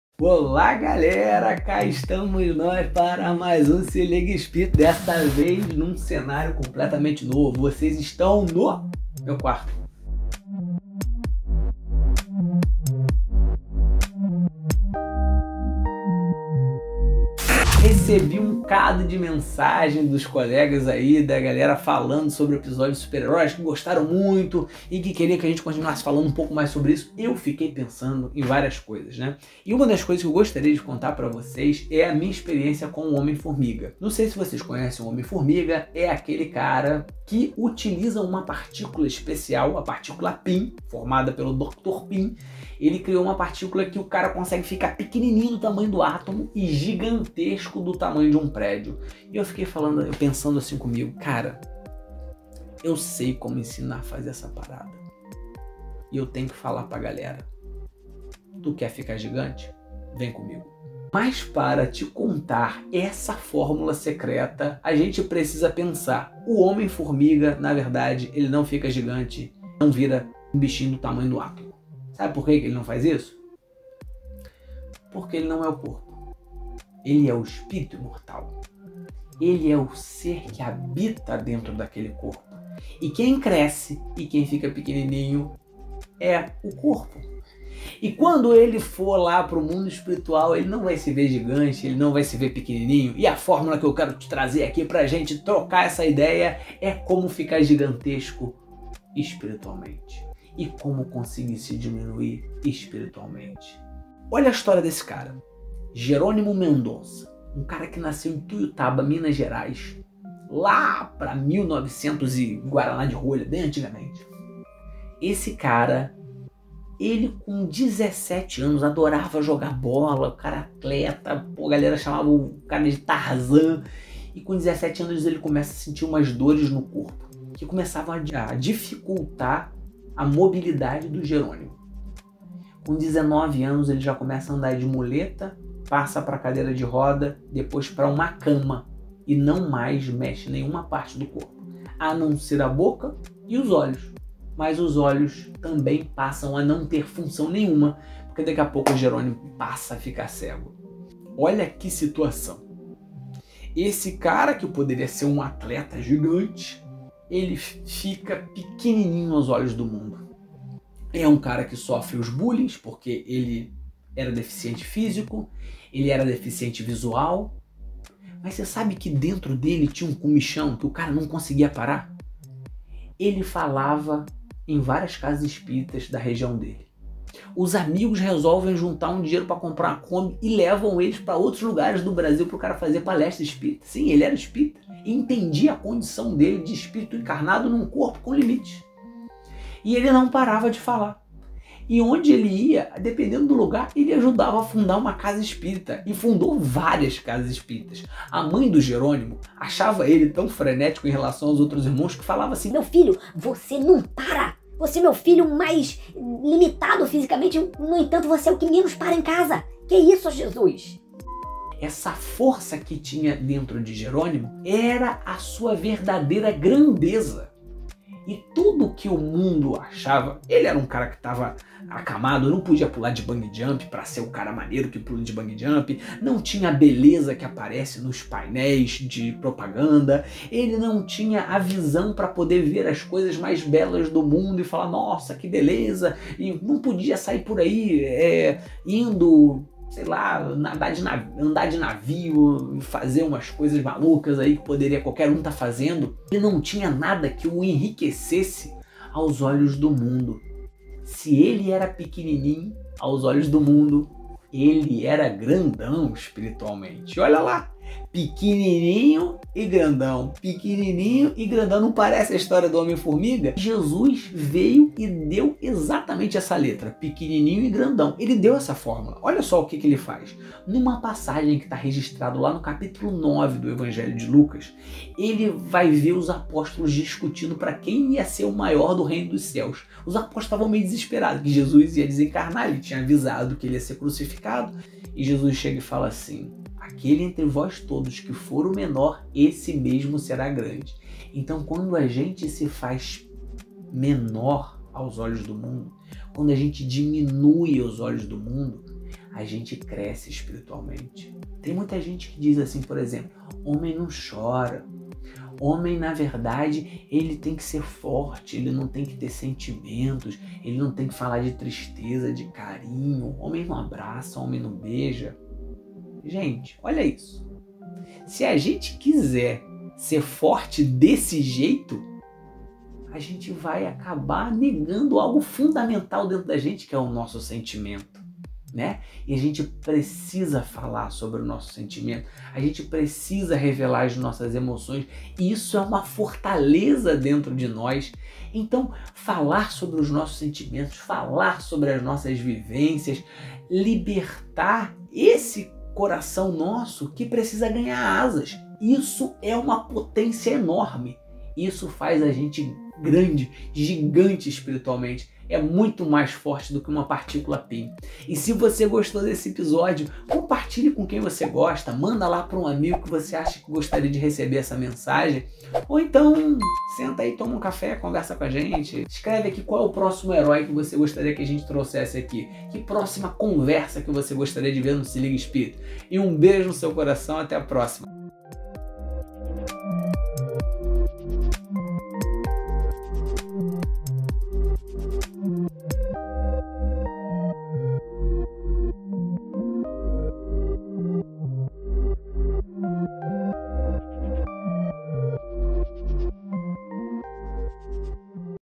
O SEGREDO DO HOMEM-FORMIGA - SE LIGA, ESPÍRITO
Você sabe qual é o segredo do Homem-Formiga? No episódio de hoje Gabriel Marins vai desvendar esse superpoder. Reúna a família e venha assistir esse episódio inédito agora mesmo.